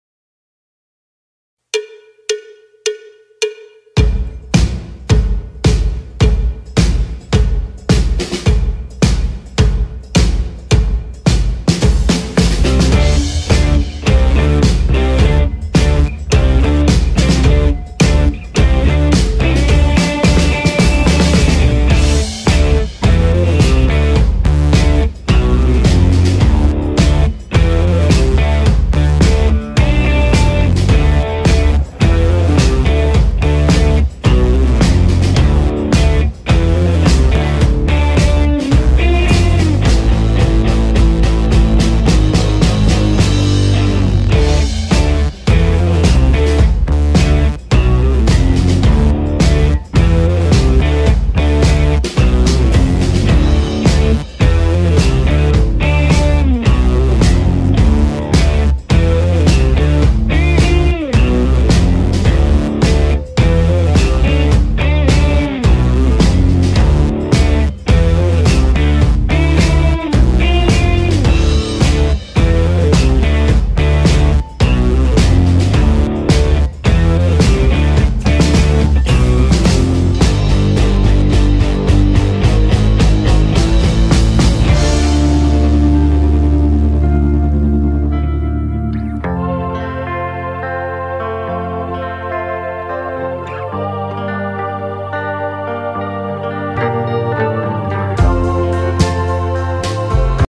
Rollingforrights Skate Protest
Rolling for Rights

Saturday September 5th San Diegans pushed from Normal Street to Balboa Park in Solidarity ✊🏿✊🏾✊🏽